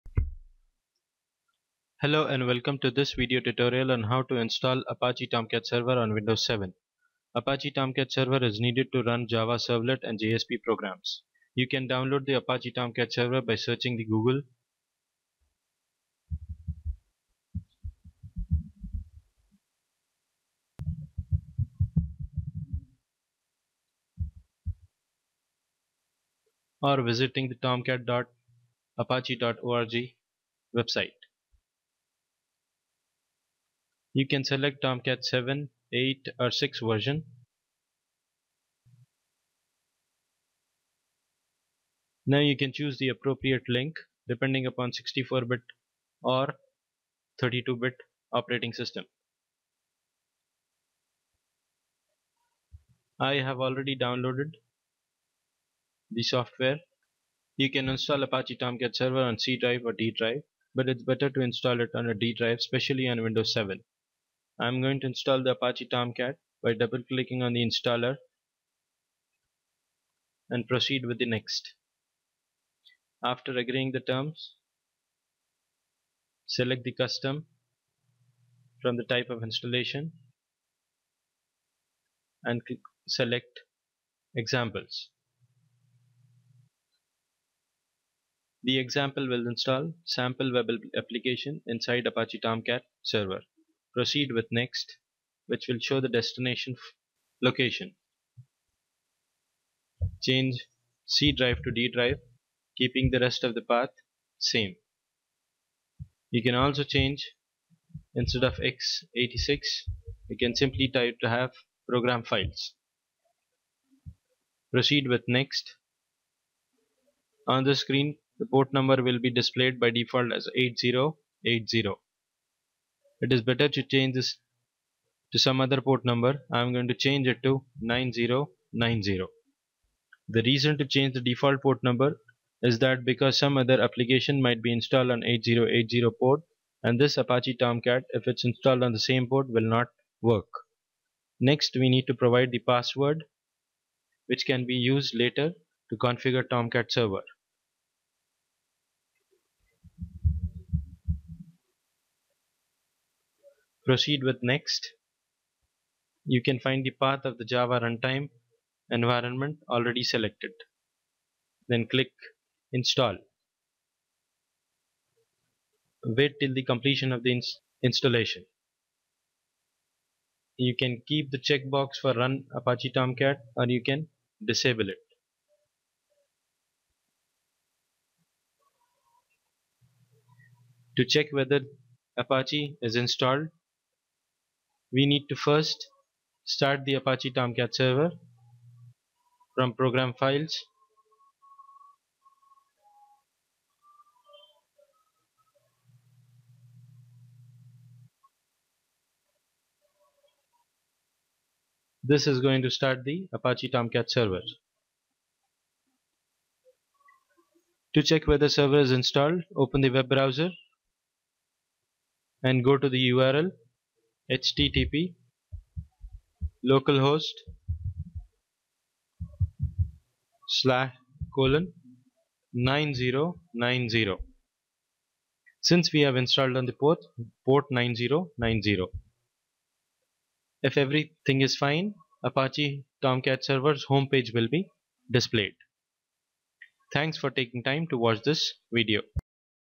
Installing Apache Tomcat Server on Windows 7
This video tutorial describes how to install Apache Tomcat Server on Windows 7. The Apache Tomcat Server is needed to run Java Servlets and Java Server Pages (JSP) programs. This video also describes the process of how to download the Apache Tomcat Server. The Apache Tomcat Server can be installed in any drive on your system. But if you are running Windows 7 64-bit operating system, then it's better to install Apache Tomcat Server on any drive other than the drive which has the operating system. The below video describes how to install Apache Tomcat Server in D drive of the system.